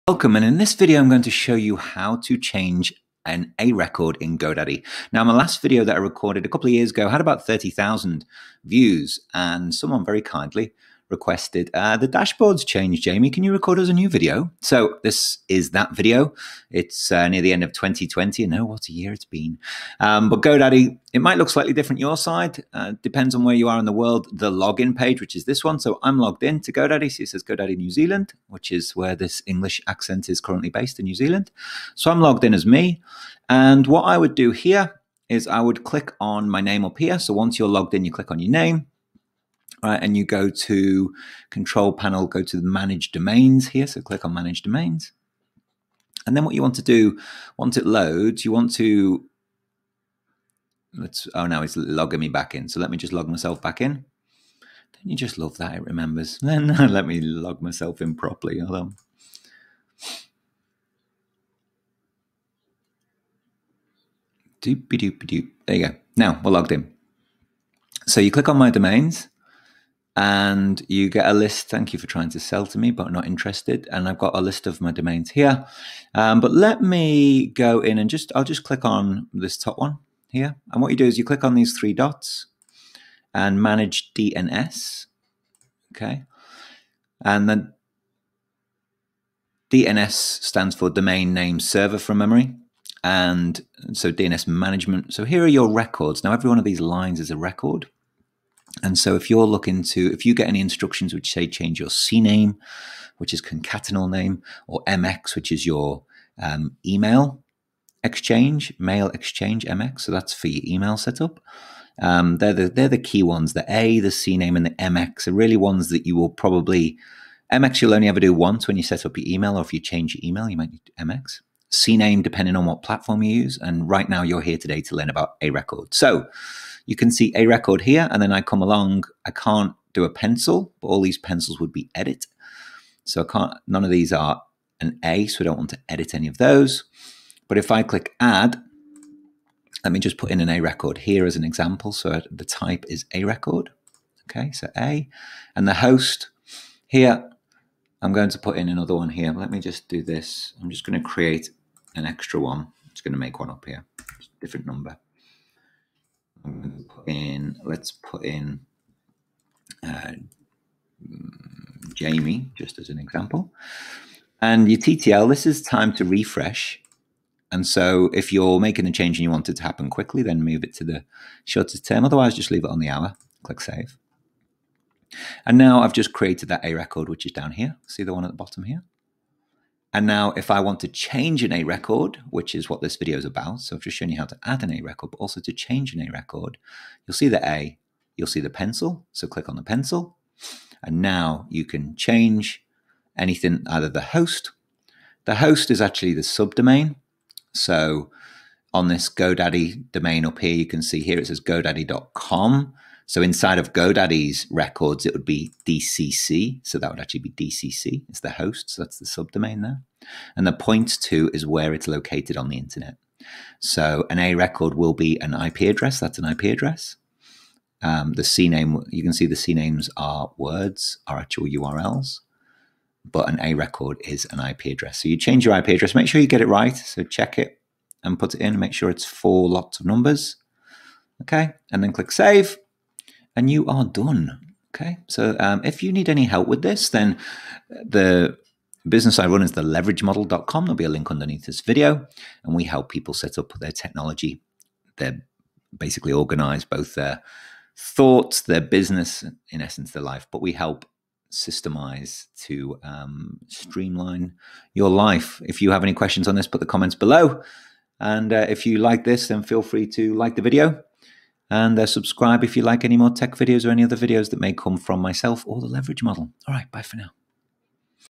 How to Change an A record in GoDaddy (Updated for 2020)
In this video, Jamie walks you through how to change an A record in Go Daddy - courtesy of